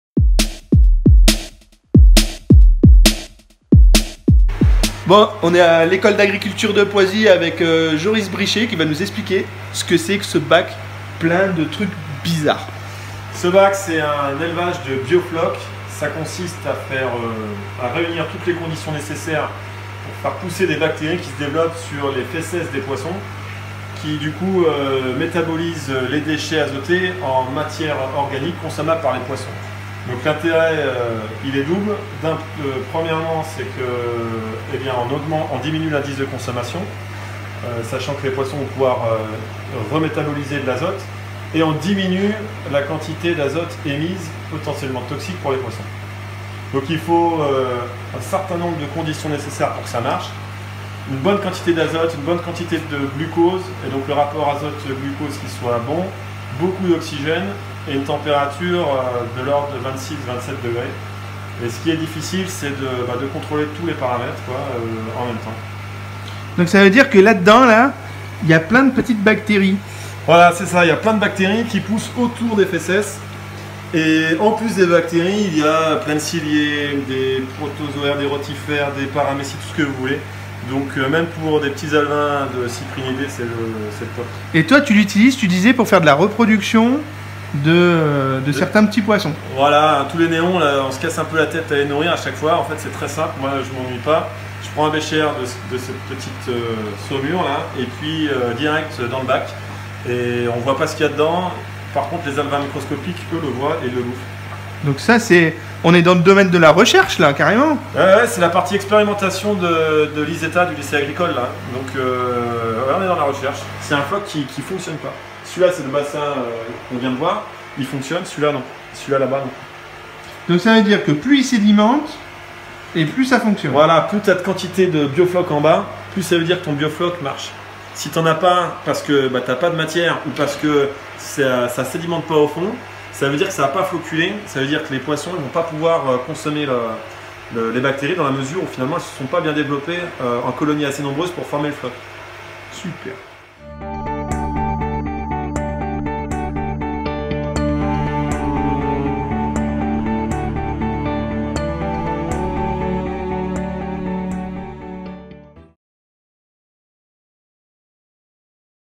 AQUACULTURE: BIOFLOC la culture expérimentale du Lycée agricole de Poisy
Petit passage sur mon ancien lieu de formation... De nouvelles méthodes aquacoles sont régulièrement testées.

La culture de Bio-Floc est l'une d'entres elles. Ce sytème d'élevage est testé car il est présente plusieurs avantages: Il est Peu gourmand en eau et améliore les performances de croissance des animaux aquatiques (Crevettes, tilapia, carpes etc...).

Ici, le Bio floc est même prélevé et utilisé pour nourrir des alevins microscopiques de poissons exotiques d'aquarium. Une méthode d'élevage qui n'a probablement pas délivré tous ces secrets...